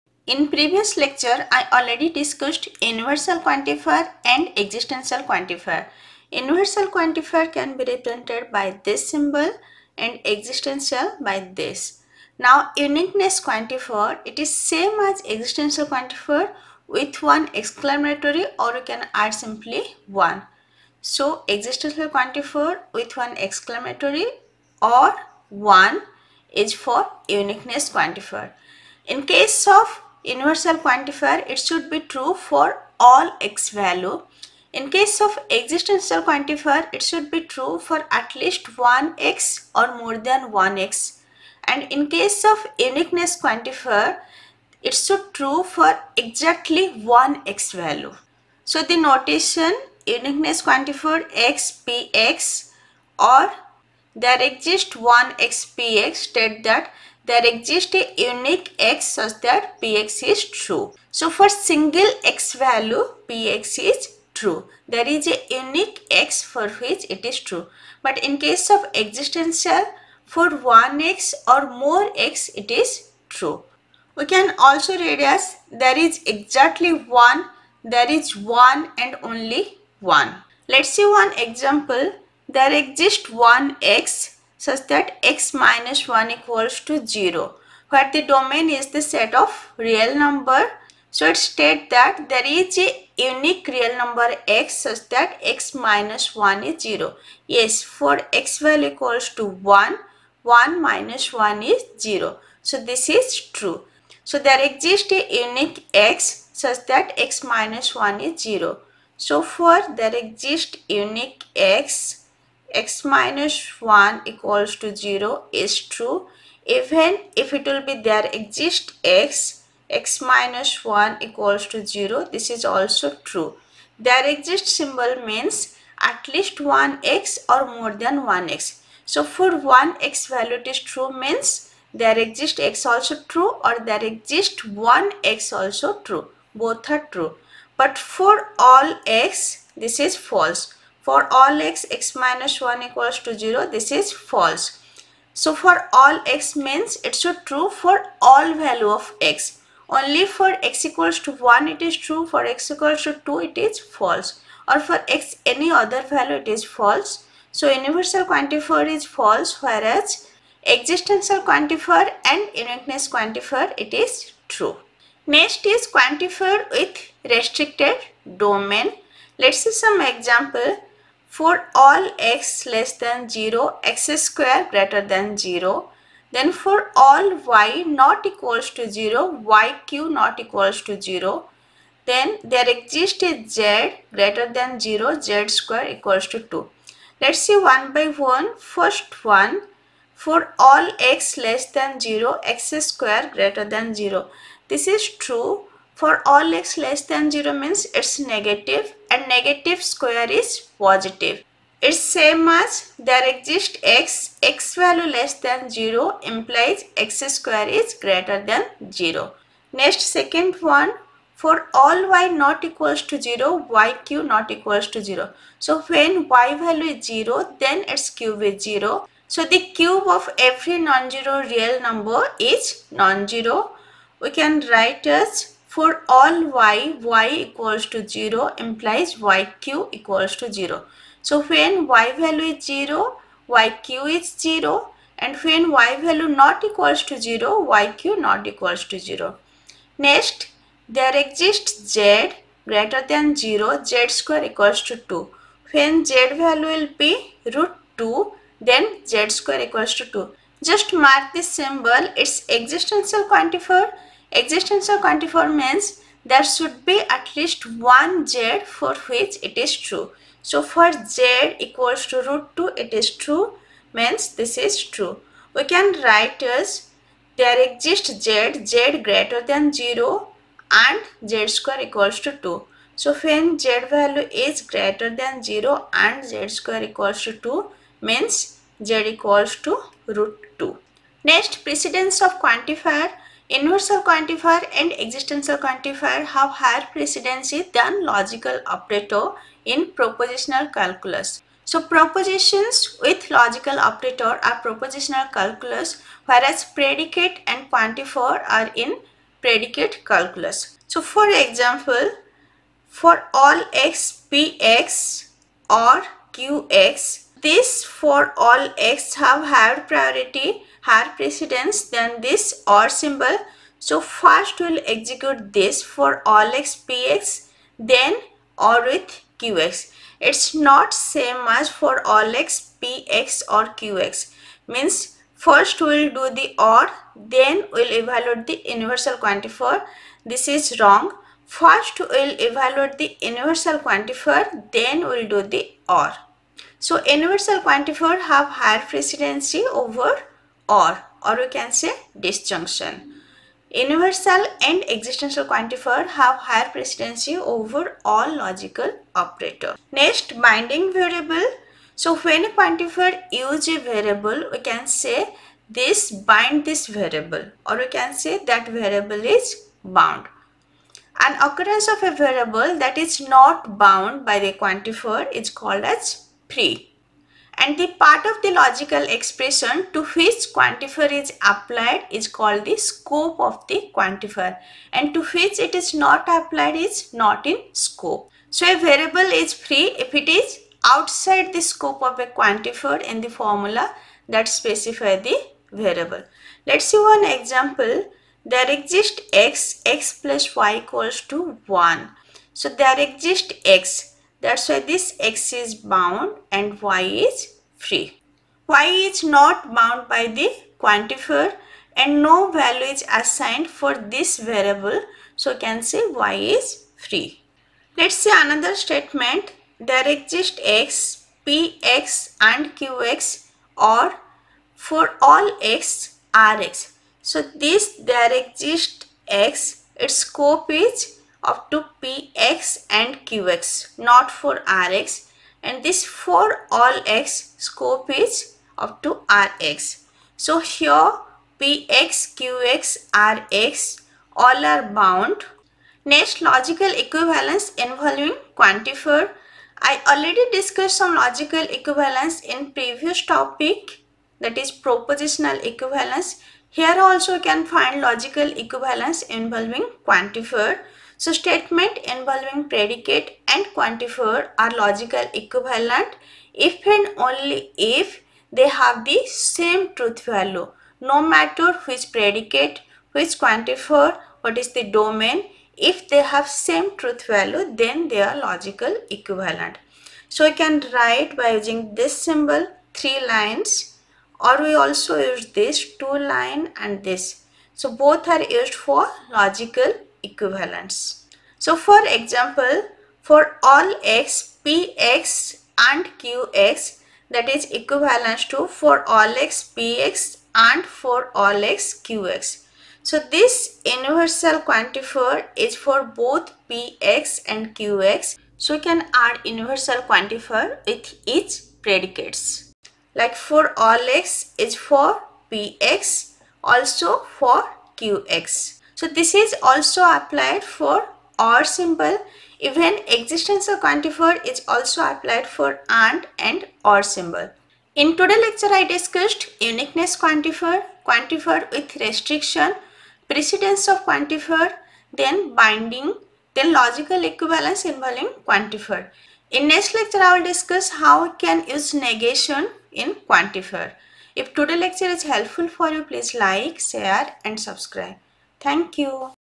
Ch 1.3.2: Logic | The Uniqueness Quantifier, Precedence of Quantifiers , Binding Variables
In this Lecture i discussed
0:12 The Uniqueness Quantifier
2:44 Quantifiers with Restricted Domains
4:41 Precedence of Quantifiers
5:49 Binding Variables
7:16 Logical Equivalences Involving Quantifiers